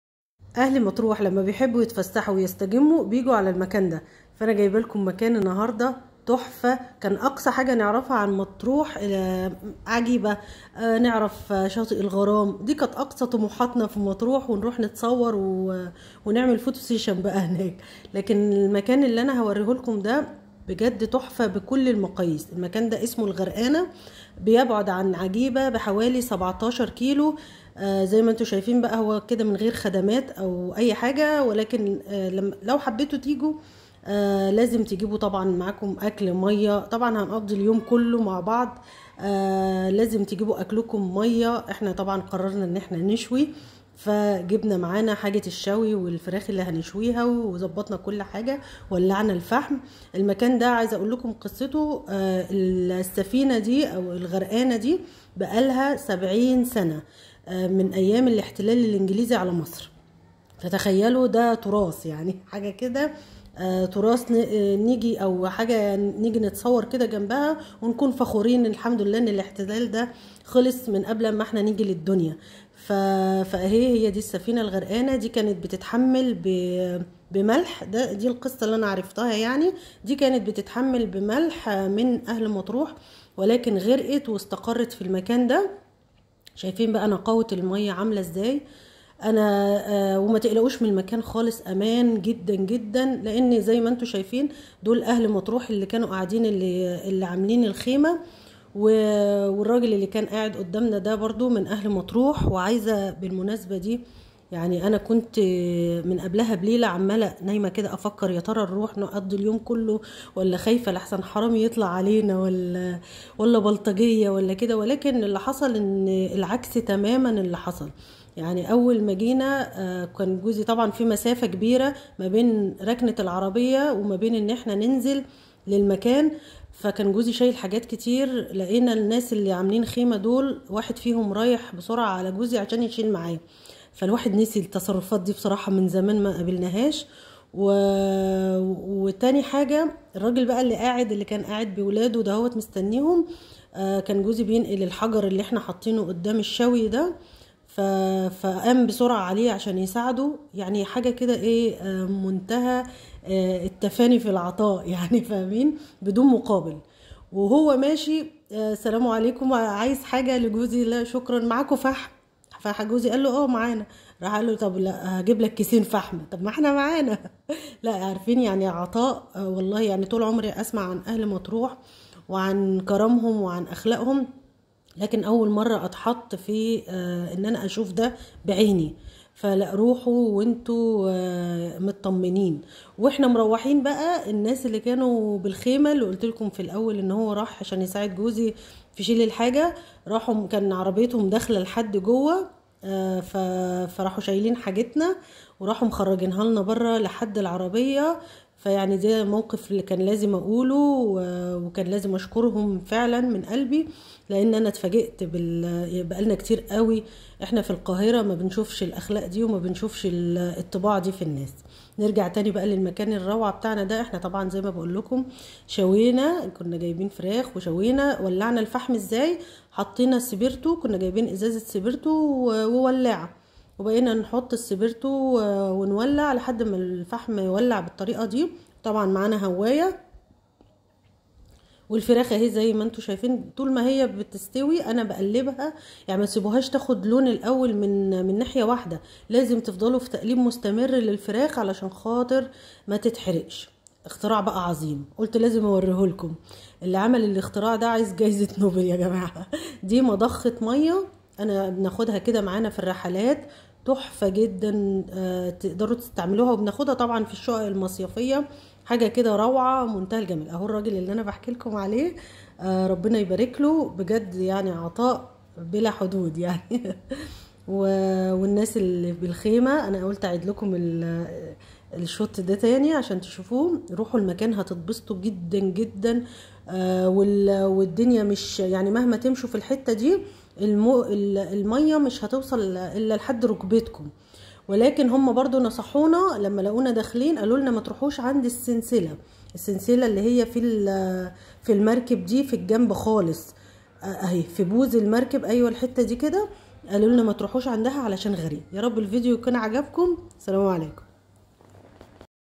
خبايا مرسى مطروح ..مكان لازم تروحوه وتستجموا فيه
اهل مطروح لما بيحبوا يتفسحوا ويتبسطوا بيروحوا المكان الروعة ده ..لازم كلكم تروحوه وتستمتعوا بيه